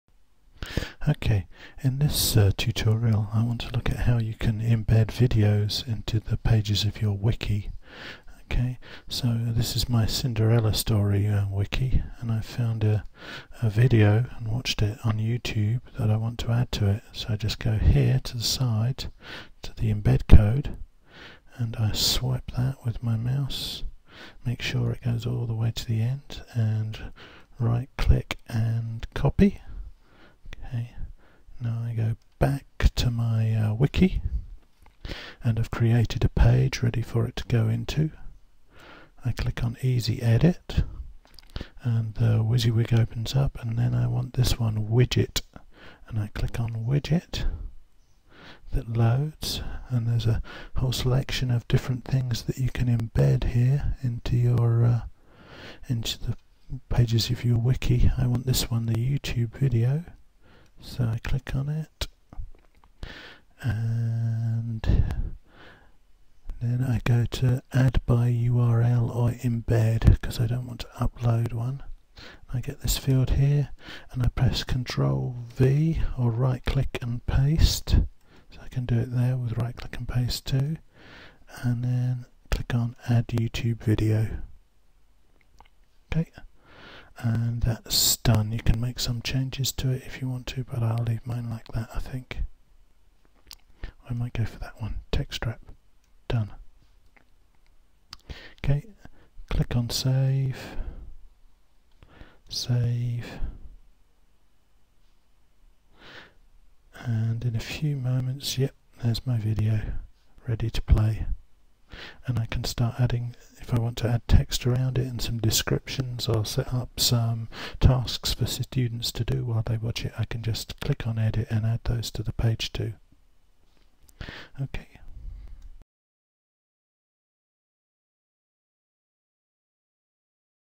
How Embed video in a wiki
This tutorial shows how to embed a video in a Wetpaint wiki